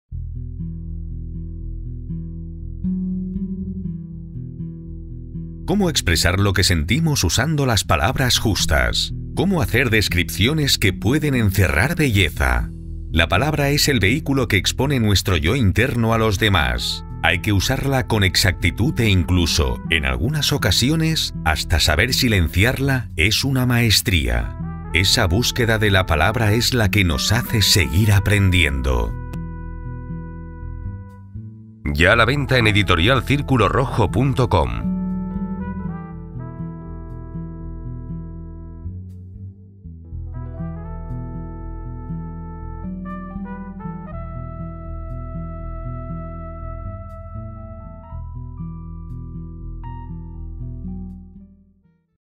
La aprendiza de la palabra (Booktrailer) - Editorial Círculo Rojo
Son reflexiones, pensamientos y hasta descripciones del cúmulo de sentimientos que, en ocasiones rodea nuestra vida y que, a veces, cuesta expresar. La búsqueda de LA PALABRA adecuada por medio (en este caso) de rimas, es lo que motiva el título de la obra, en resumen, siempre aprendiendo. “Después de acaecida la batalla sólo quedan las palabras”

Ángela Hernández Benito
(Responsable de programación de la Casa José Zorrilla de Valladolid)